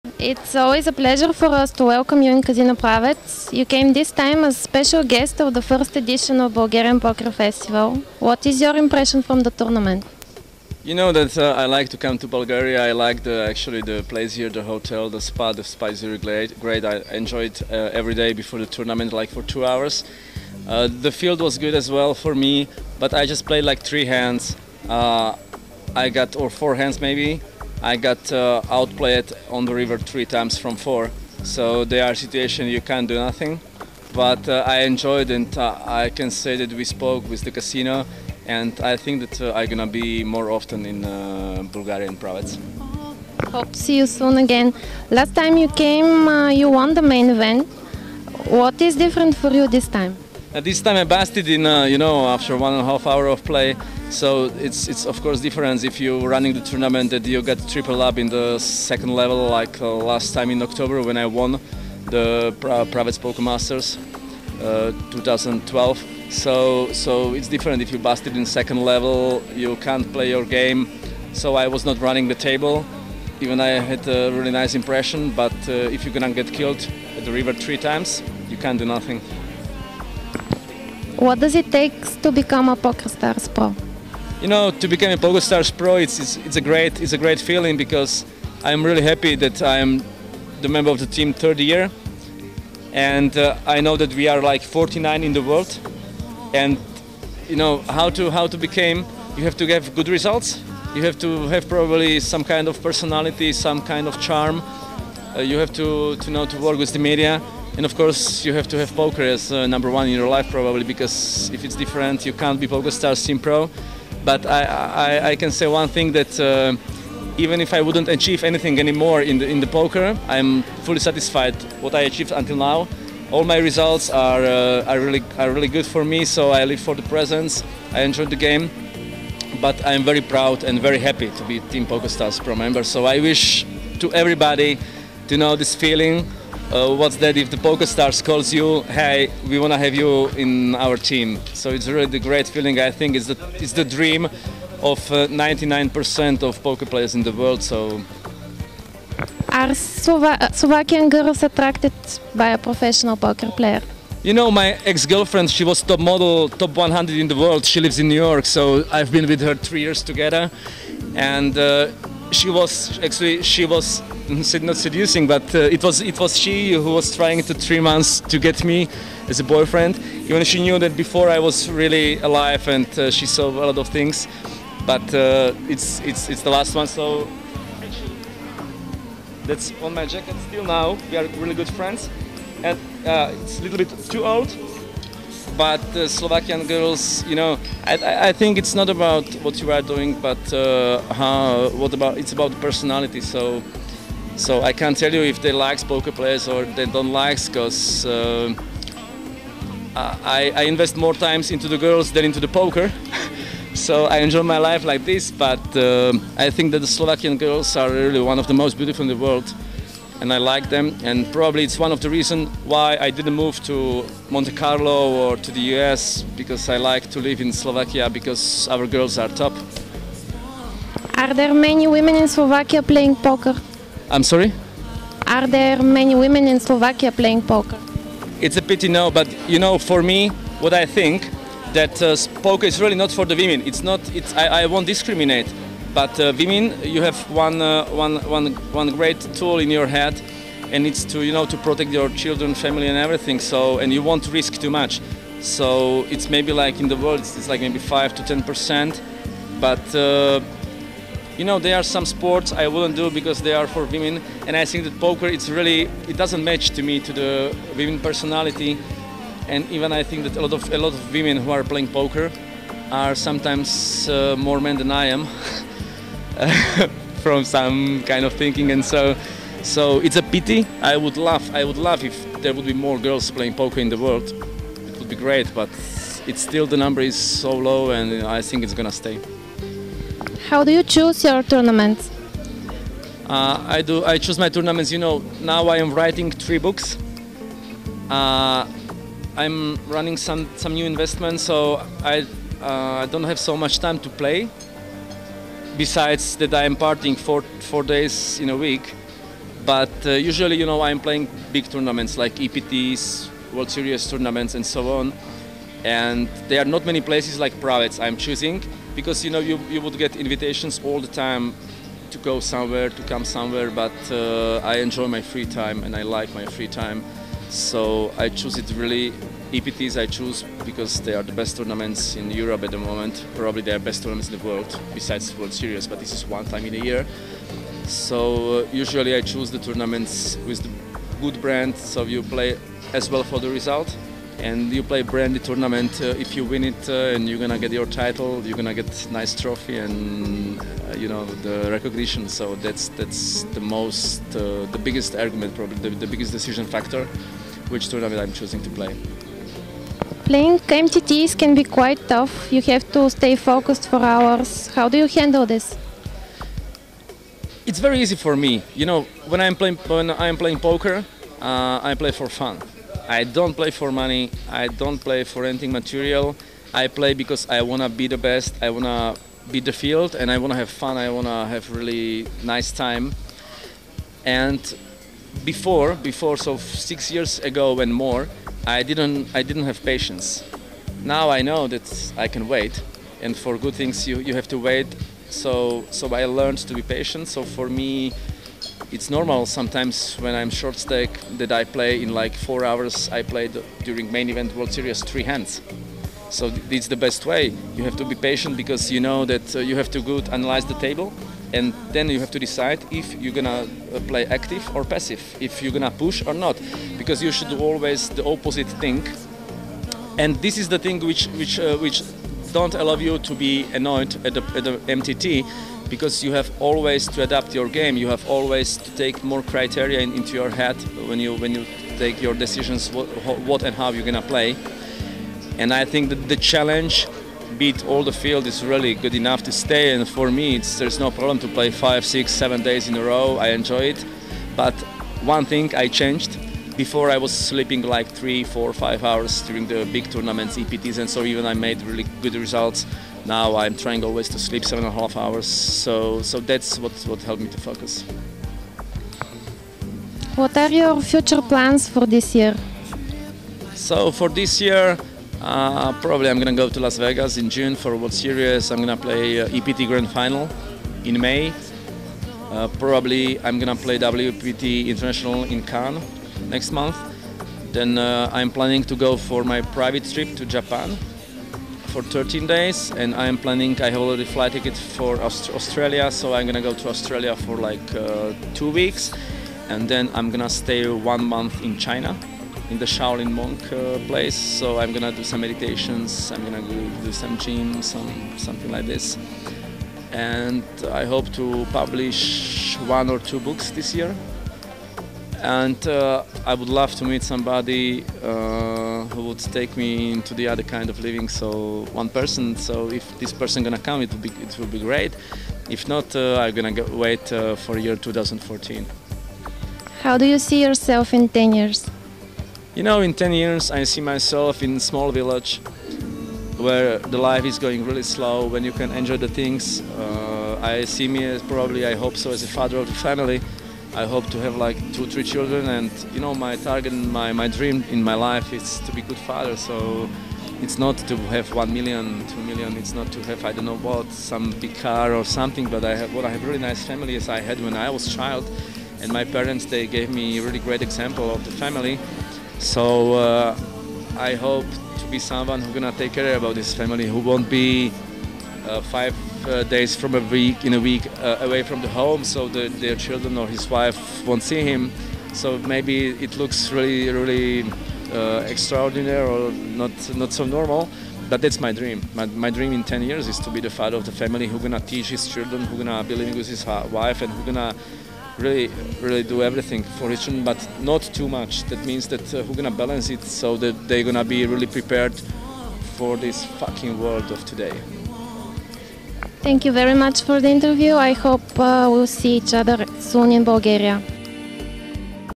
Dag Palovic interview
Интервю с Даг Павлович - Team Poker Stars Pro от Българския Покер Фестивал, Правец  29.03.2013г.